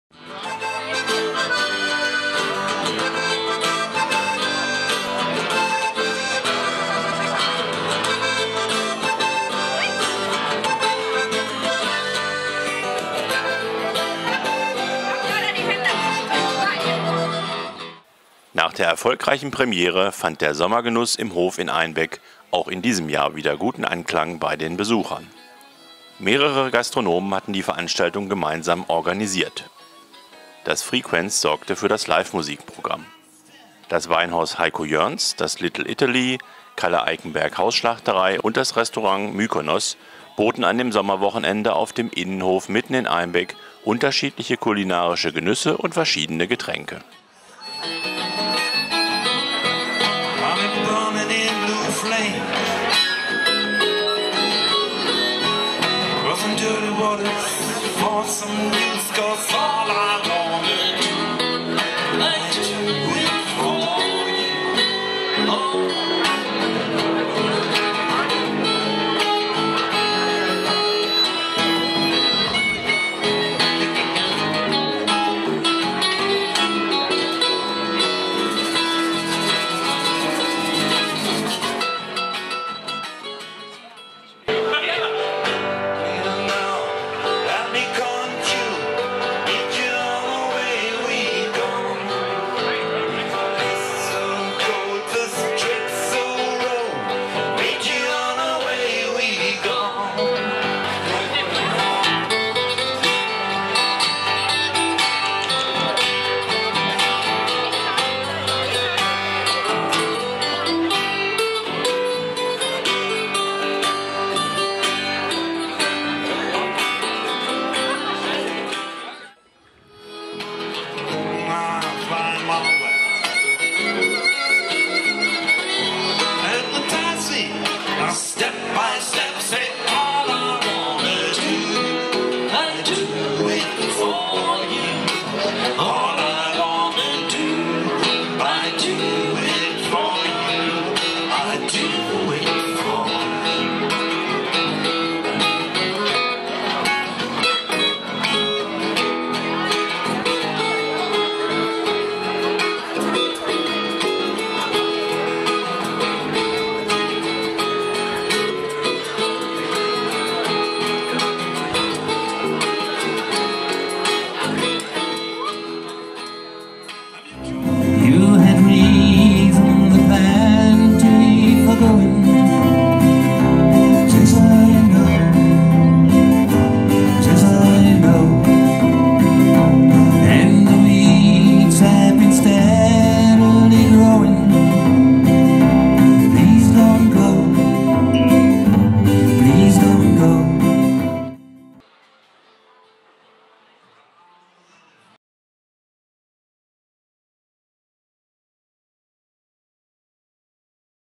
Sommer-Genuss im Hof in Einbeck fand 2016 wieder guten Anklang
Nach der erfolgreichen Premiere fand der "Sommer-Genuss im Hof" in Einbeck auch am 9./10. Juli 2016 wieder guten Anklang bei den zahlreichen Besuchern. Mehrere Gastronomen hatten die Veranstaltung gemeinsam organisiert, das "FreeQuenz" (Jürgen Harburg) sorgte für das Live-Musik-Programm. Das Weinhaus Heiko Jörns, das Little Italy, Kalle Eikenberg Hausschlachterei und das Restaurant Mykonos boten an dem Sommer-Wochenende auf dem Innenhof mitten in Einbeck unterschiedliche kulinarische Genüsse und verschiedene Getränke.